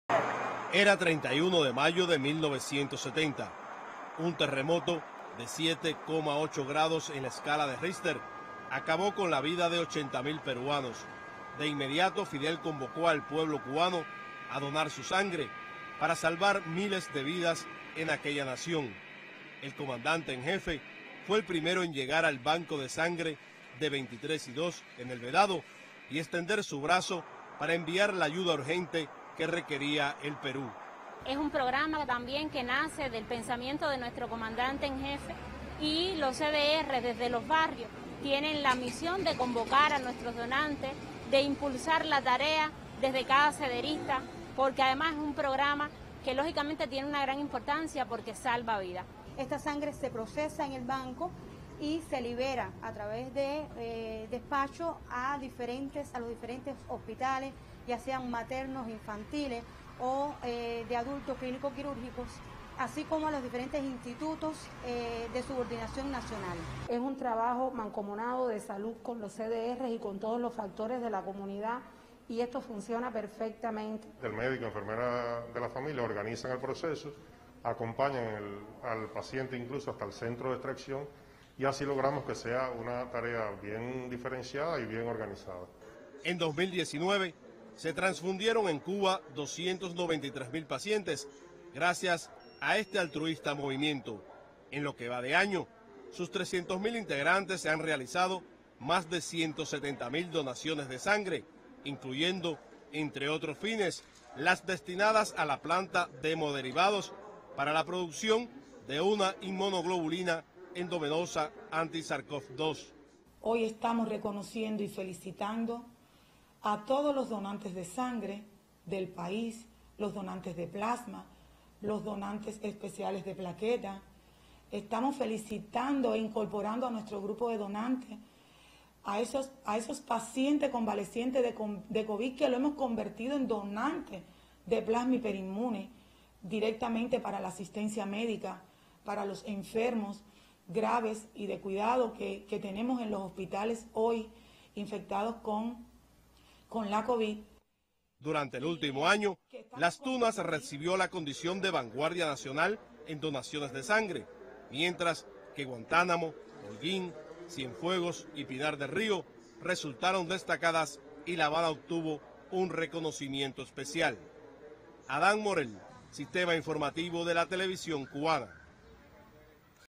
Comienza en Cuba jornada Nacional del Donante de Sangre
La jornada Nacional del DonanteDeSangre se inicia este sábado en toda Cuba. En esta ocasión se rinde tributo a FidelCastro, quien realizara, hace medio siglo, la primera donación , la cual dedicó al pueblo peruano tras el terremoto que devastó a esa nación.